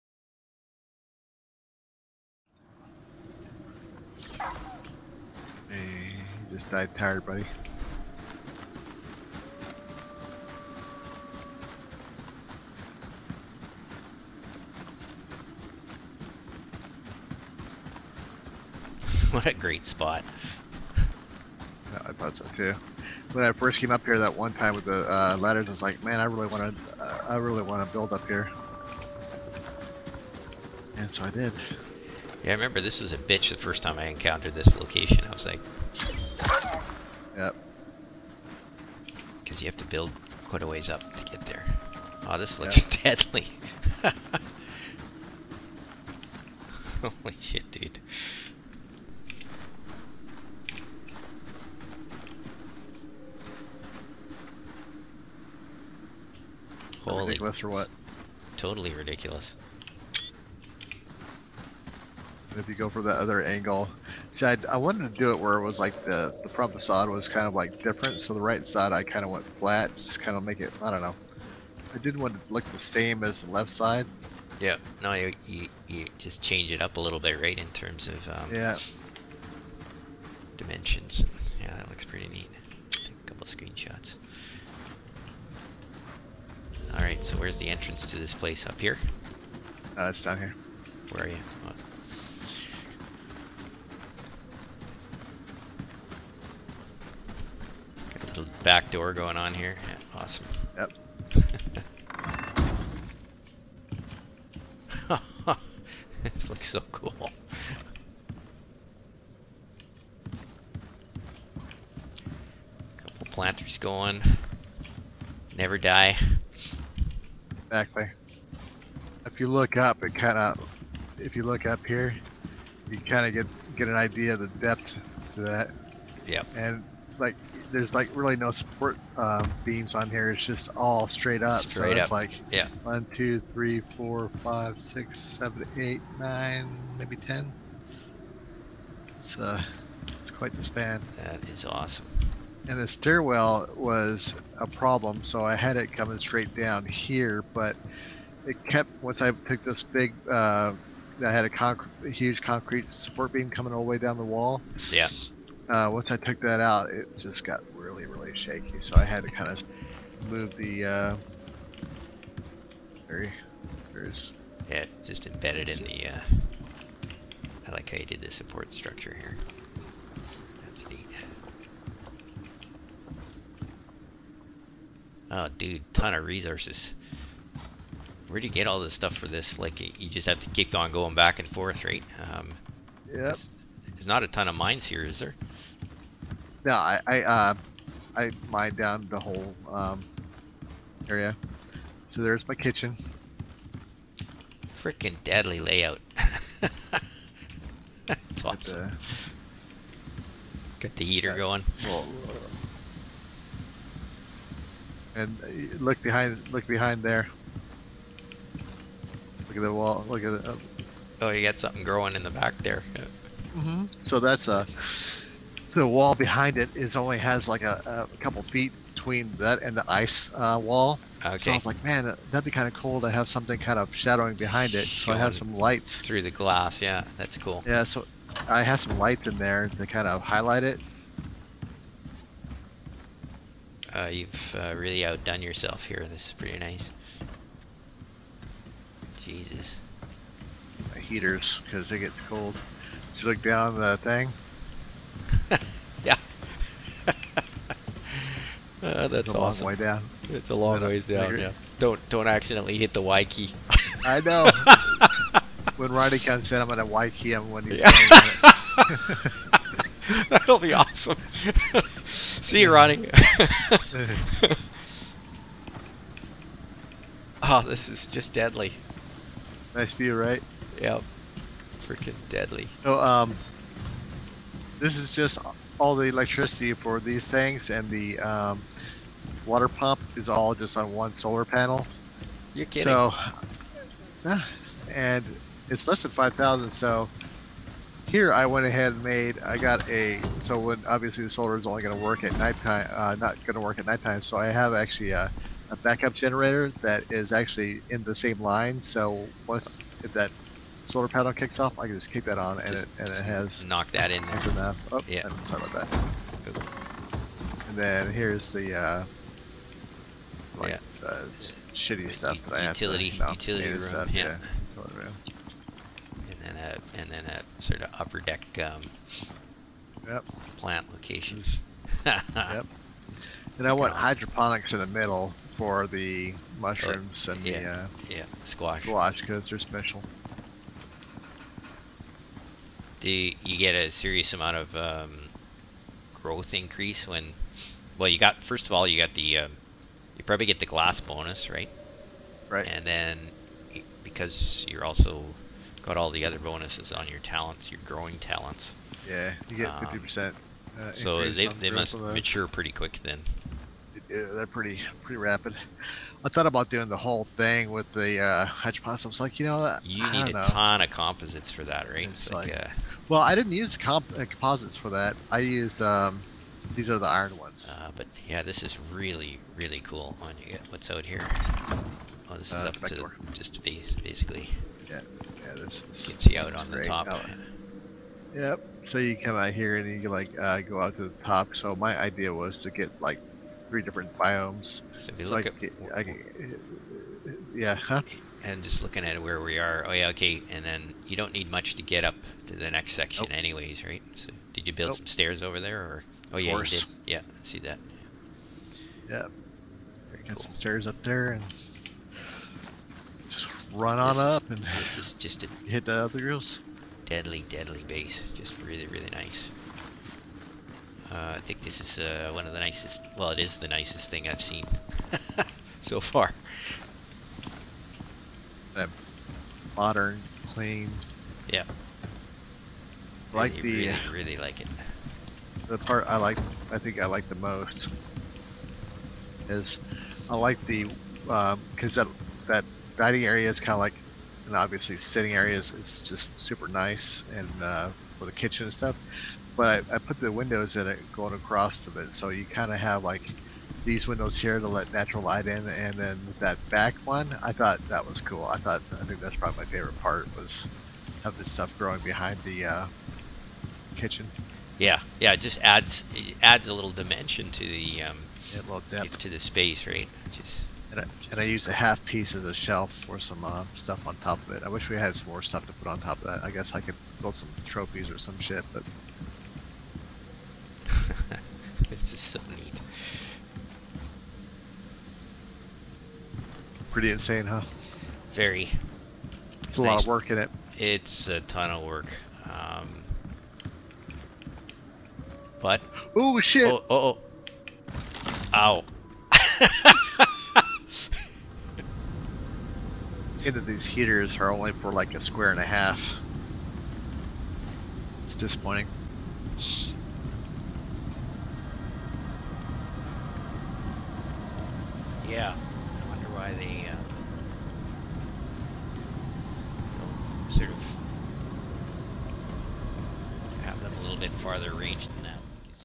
Ice Palace
Welcome to the Ice Palace!
A video tour of an awesome base created by Liquidjesus in the artic biome of the Styx map.